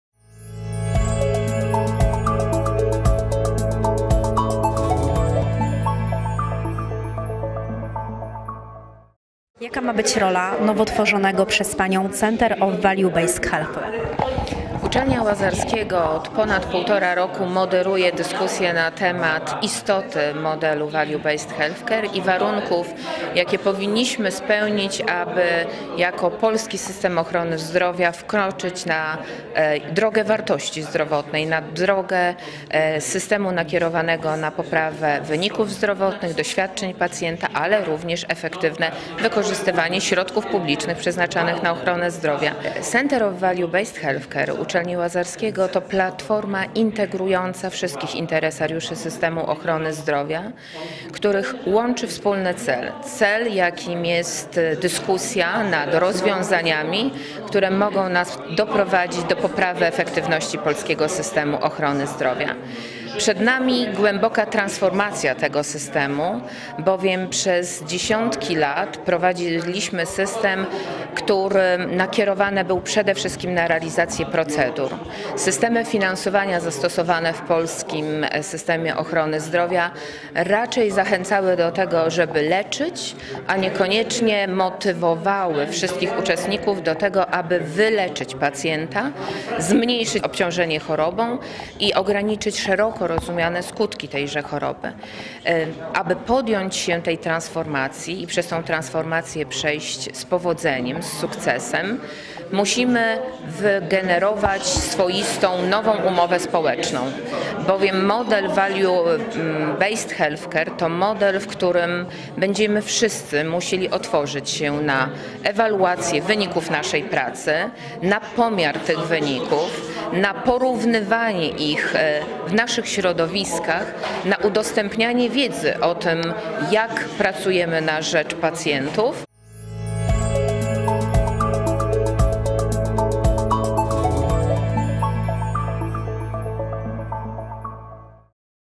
Małgorzata Gałązka-Sobotka: Przed nami głęboka transformacja systemu ochrony zdrowia
- Dyskutujemy nad rozwiązaniami, które mogą nas doprowadzić do poprawy efektywności polskiego systemu ochrony zdrowia - powiedziała Medexpressowi dr n. ekon. Małgorzata Gałązka-Sobotka, dyrektor Instytutu Zarządzania w Ochronie Zdrowia Uczelni Łazarskiego.